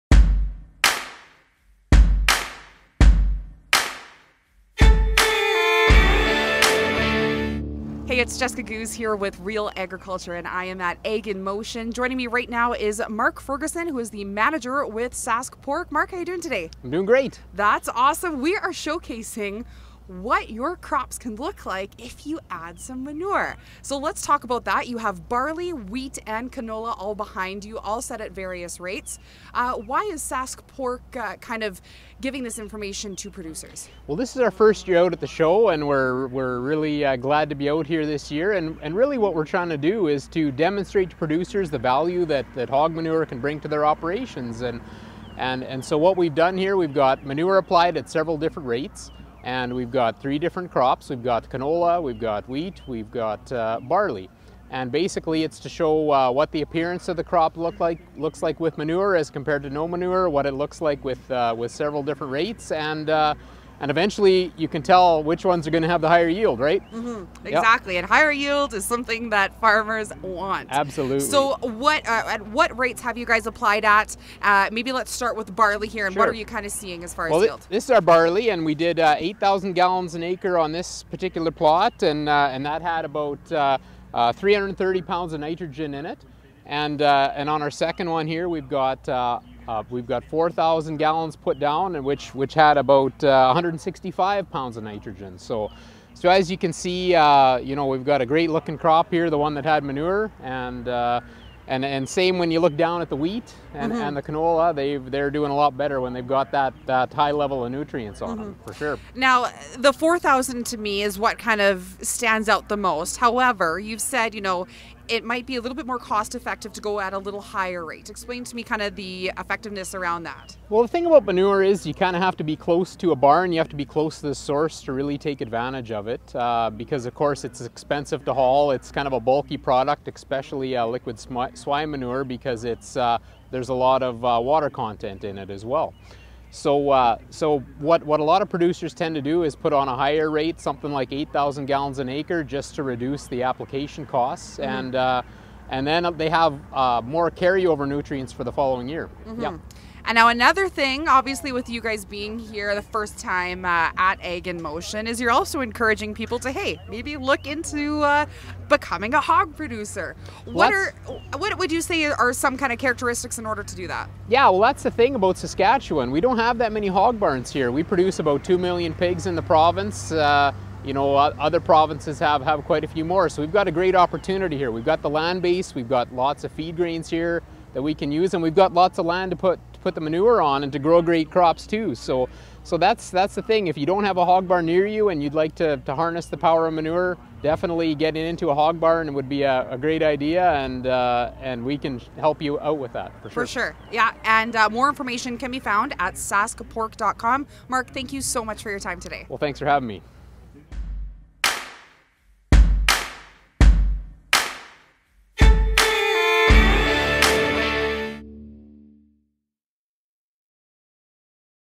The power of poo: Adding manure to the crop production mix
Mark Ferguson, with SaskPork, was on hand at Ag in Motion, near Saskatoon, Sask., to demonstrate the value of hog manure to barley, wheat, and canola crops, looking at none vs. some vs. lots of manure. The visual difference, for those who toured the plots, was striking.